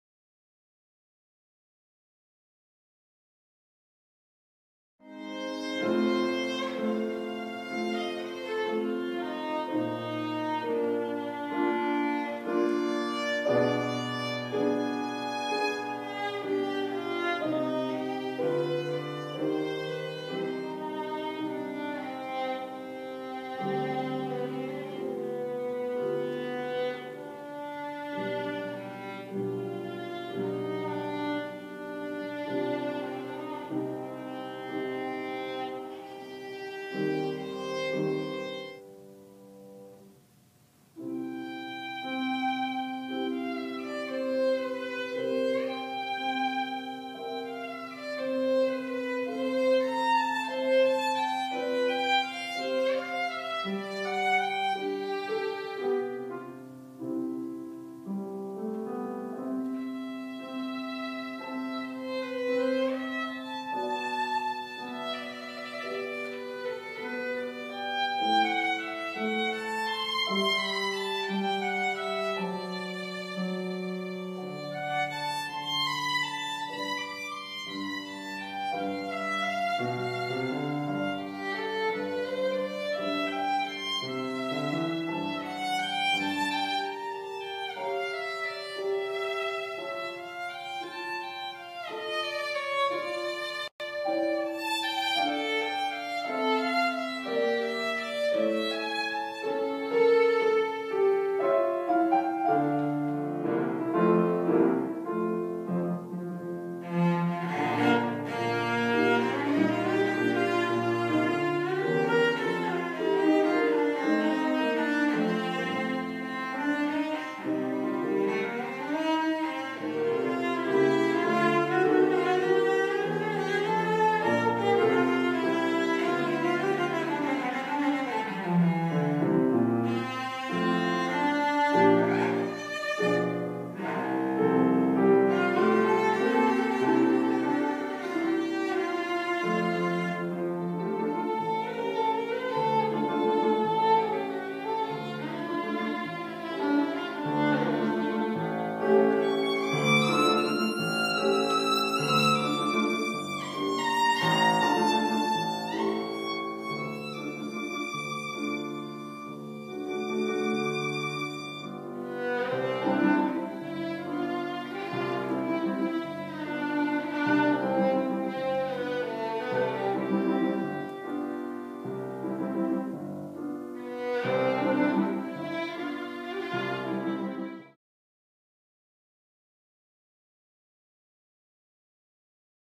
Virtuoso Program Solo Recitals
San Domenico School Virtuoso Program spring string solo recital concert held February 15, 2012.